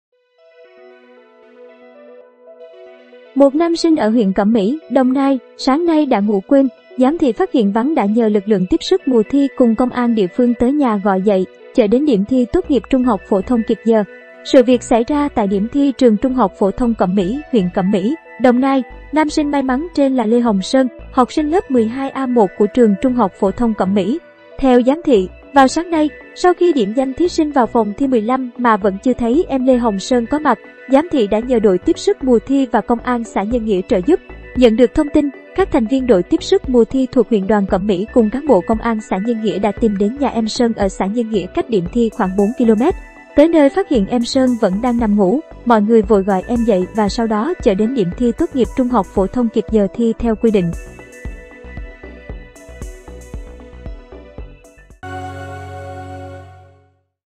Thí sinh ngủ quên, đội Tiếp sức mùa thi đến nhà chở đi thi tốt nghiệp THPT
Một nam sinh ở H.Cẩm Mỹ (Đồng Nai) sáng nay đã ngủ quên, giám thị phát hiện vắng đã nhờ lực lượng Tiếp sức mùa thi cũng Công an địa phương tới nhà gọi dậy, chở đến điểm thi tốt nghiệp THPT kịp giờ.